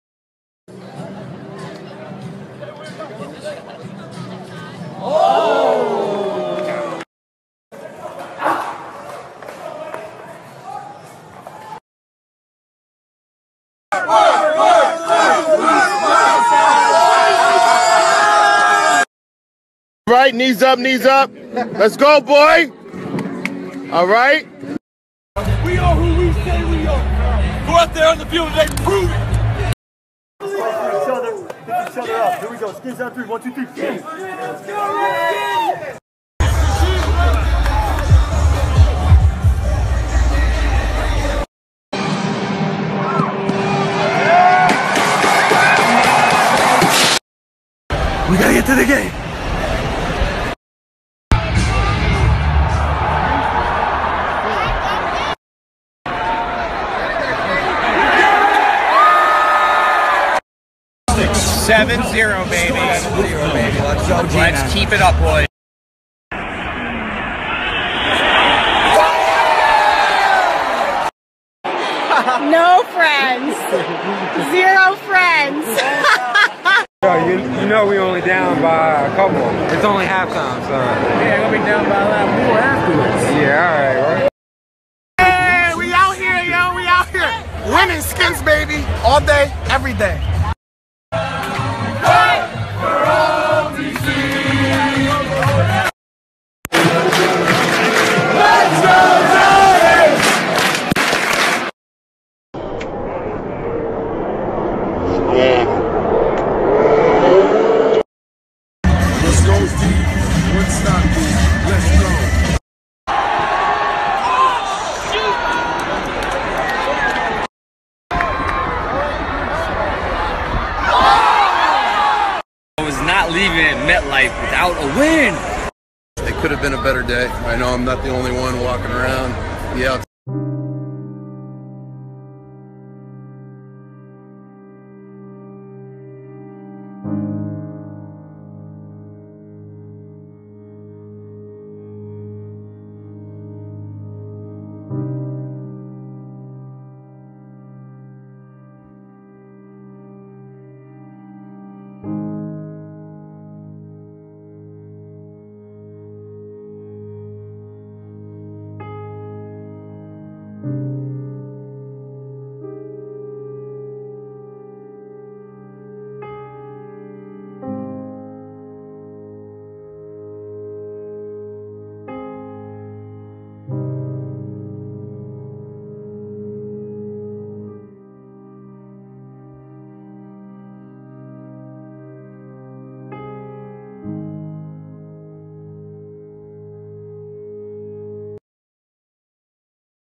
Odell Beckham Jr Cries After Getting Beat By Josh Norman And Washington Redskins
Odell Beckham Jr Cries After Getting Beat By Josh Norman And Washington Redskins - YouTube